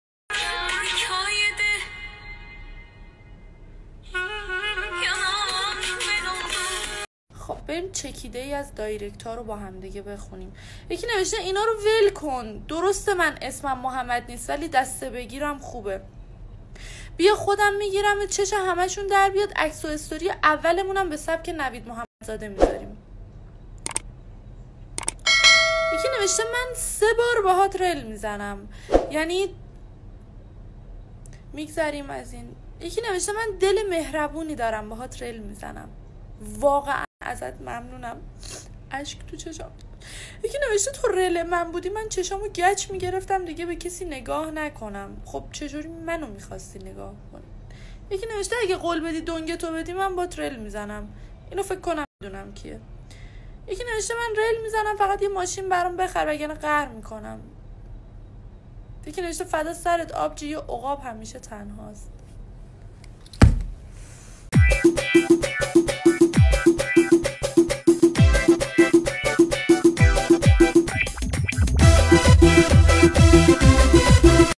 طنین میخواد رل بزنه کی حاضره؟؟ 😍😂 کلیپ خنده دار طنین حقیقی
😂😂 واکنش ملت همیشه در صحنه نسبت به رل زدن طنین
😎 کلیپ طنز طنین رو از دست ندید 😎